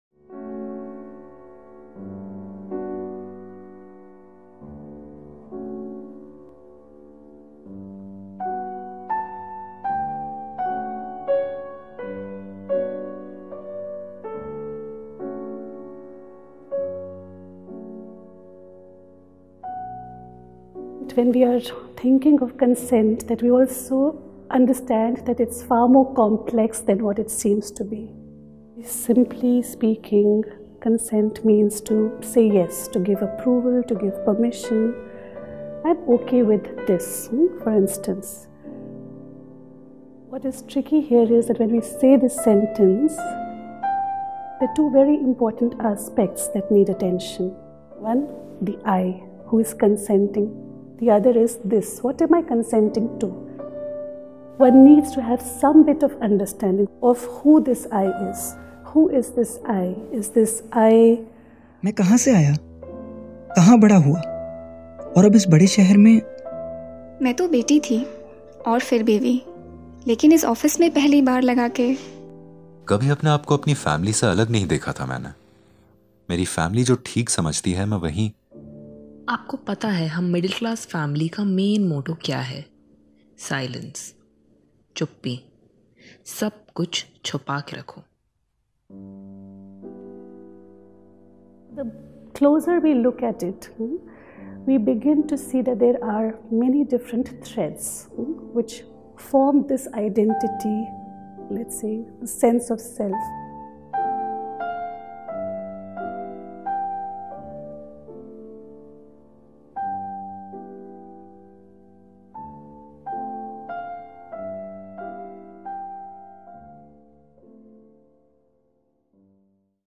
CHAPTER 2 - The ‘I’ in Consent
Who is the ‘I’ that gives consent? What makes that self?

How does your context, your history, play into your ability to negotiate consent?

How important is it to know where you come from, to understand where you are going?